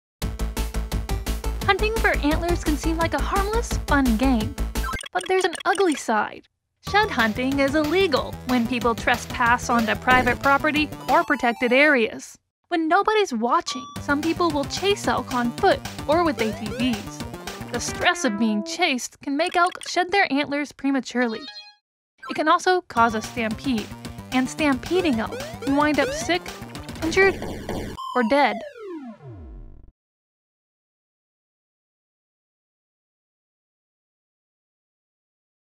Why Some People Illegally Chase Down Elk With ATVs
Law enforcement have caught out-of-season hunters using ATVS to chase elk and steal the animal's antlers.

Written, Produced & Narrated by Courtney Flatt

Animation by MacGregor Campbell and John Rosman
Sound Effects from Freesound.org: cabeeno-rossley, ls, willy-ineedthatapp-com, kwahmah-02, timgormly, adamweeden, themusicalnomad, jivatma07, taira-komori